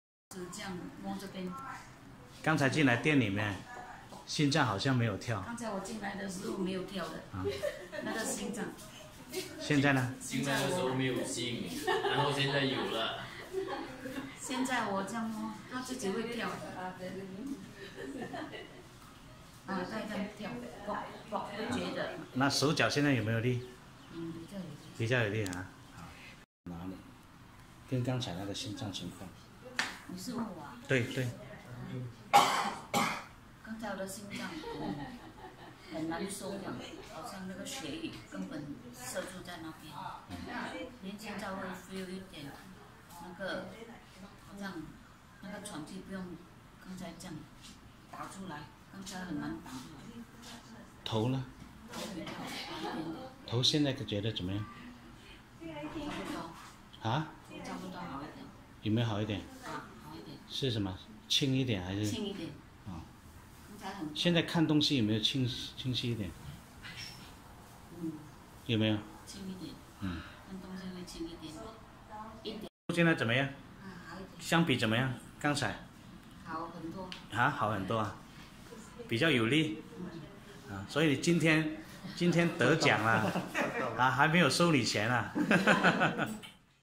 Testimonial after Pasting the Quantum chips and Cosmic Energy Pad
This Aunty came into Shop saying her heart is weak n she hardly can feel her heart beating. After pasting the quantum n fanning her with the Cosmic Energy Pad she can feel her heart beat.